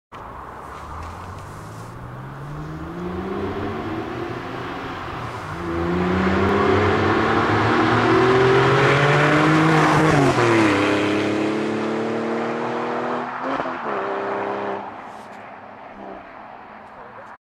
9-EVO /// SCART Exhausts Porsche 996 Carrera 3.6L
SCART Hand Made Exhausts: Porsche 996 3.6L Sport Exhaust SCART Ultima Recital

Silencieux Ultima récital pour Porsche 996

Extérieurement identique à la version SCART classic, cette version est l'ultime aboutissement d'une série de 15 prototypes.

Les échappements SCART propose cette version ULTIMA RECITAL qui fait autorité dans le monde Porsche.

VIDEO !!! Cette version apporte un grain de folie supplémentaire dans les variation de régime par son système ULTIMA. 🤩🔥